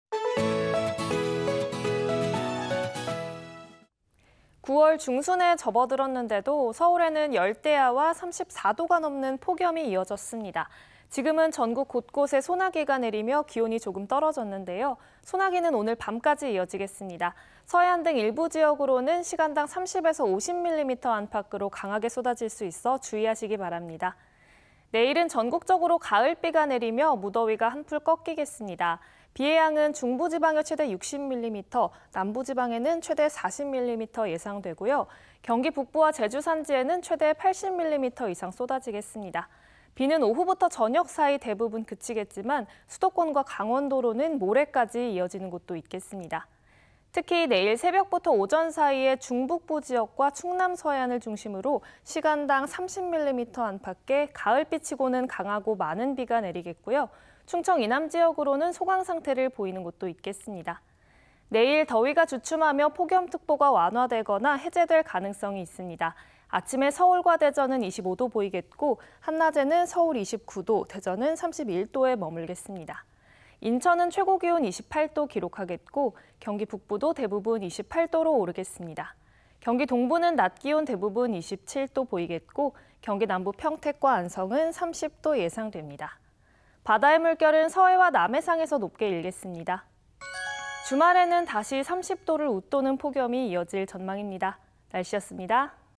[9월 12일 날씨] 9월 낮더위 폭염 절정… 가을비가 내리며 무더위 약해져요 (문지영 기상캐스터) / 오늘의 날씨
【 기상캐스터 】

9월 중순에 접어들었는데도 서울에는 열대야와 34도가 넘는 폭염이 이어졌습니다.

지금은 전국 곳곳에 소나기가 내리며 기온이 조금 떨어졌는데요, 소나기는 오늘 밤까지 이어지겠습니다. 서해안 등 일부지역으로는 시간당 30~50mm 안팎으로 강하게 쏟아질 수 있어 주의하시기 바랍니다.

내일은 전국적으로 가을비가 내리며 무더위가 한풀 꺾이겠습니다. 비의 양은 중부지방에 최대 60mm, 남부지방에는 최대 40mm 예상되고요. 경기북부와 제주산지에는 최대 80mm 이상 쏟아지겠습니다. 비는 오후부터 저녁 사이 대부분 그치겠지만, 수도권과 강원도로는 모레까지 이어지는 곳도 있겠습니다.

특히 내일 새벽부터 오전 사이에 중북부지역과 충남서해안을 중심으로 시간당 30mm 안팎의, 가을비치고는 강하고 많은 비가 내리겠고요. 충청이남지역으로는 소강상태를 보이는 곳도 있겠습니다.

내일 더위가 주춤하며 폭염특보가 완화되거나 해제될 가능성이 있습니다.
아침에 서울과 대전 25도 보이겠고, 한낮에는 서울 29도, 대전 31도에 머물겠습니다.
인천은 최고기온 28도 기록하겠고,
경기북부도 대부분 28도로 오르겠습니다.
경기동부는 낮기온 대부분 27도 보이겠고,
경기남부 평택과 안성은 30도 예상됩니다.

바다의 물결은 서해와 남해상에서 높게 일겠습니다.

주말에는 다시 30도를 웃도는 폭염이 이어질 전망입니다. 날씨였습니다.